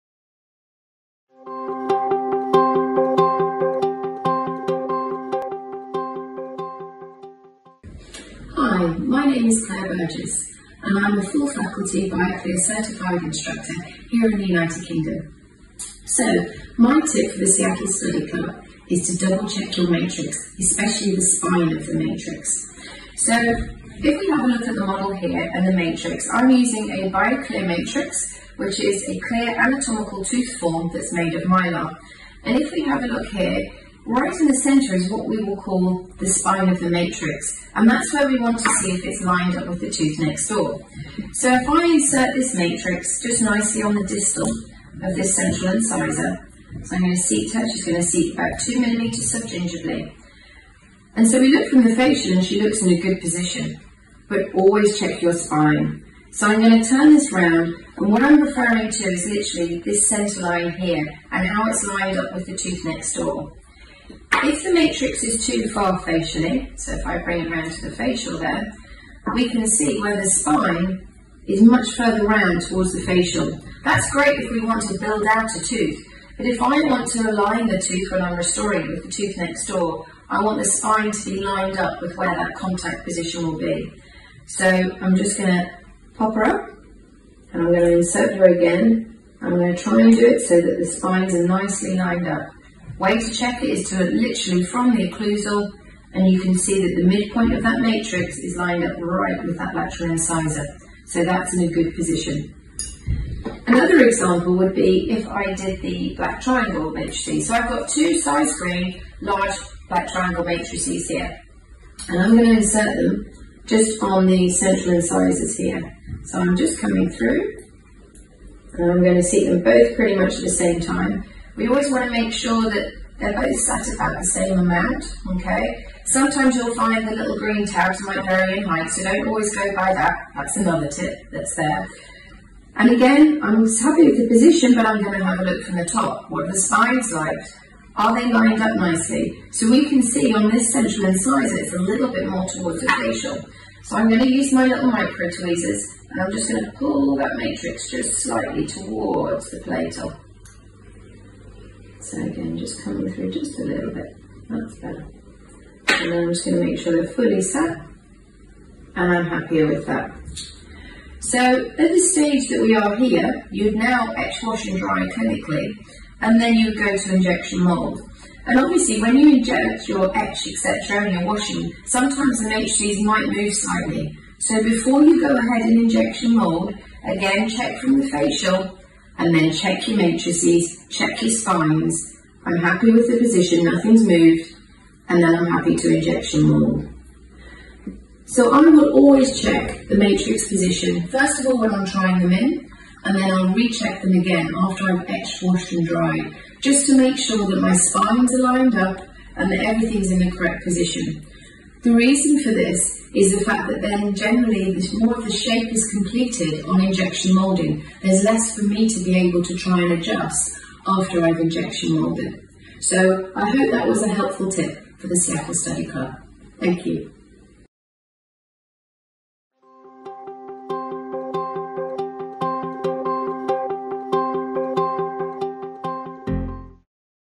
Expert Tip — Dr Claire Burgess — Checking Alignment
Dr Claire Burgess recommends checking the matrix position to ensure everything is lined up before injection molding.